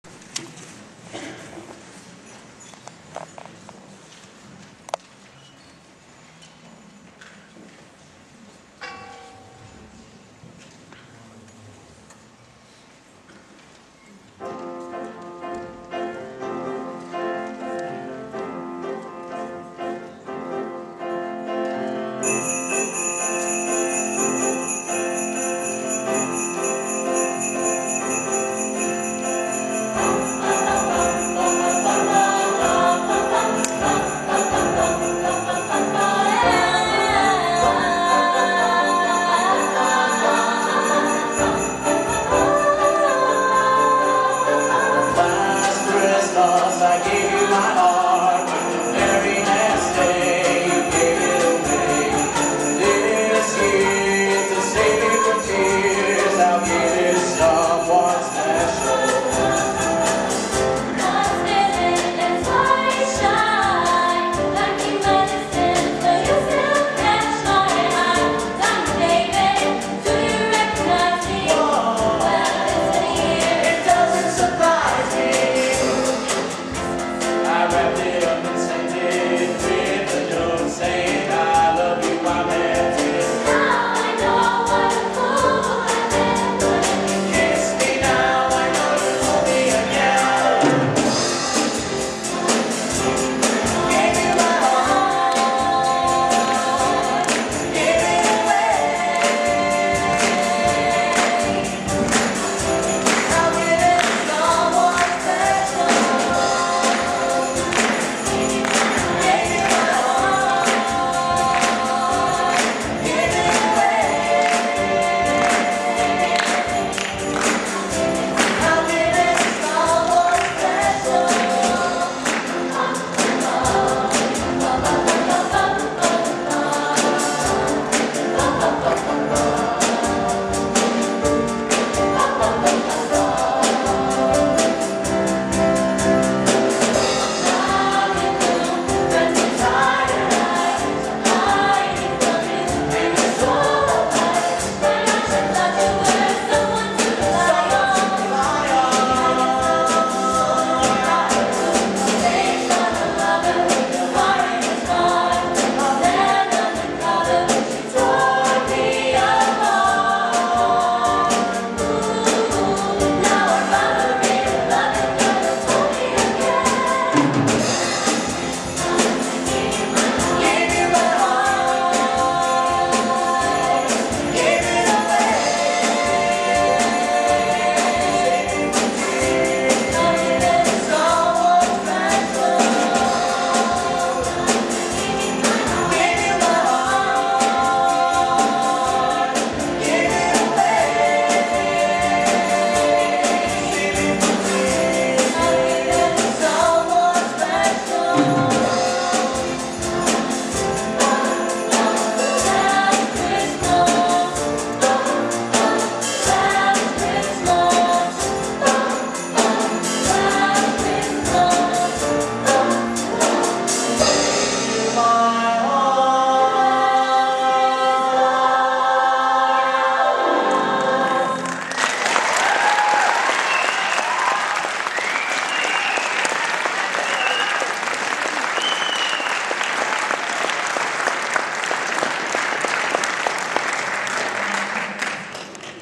MOV05C_1.MOD
Guerin Prep High School Choral recital 12-6-11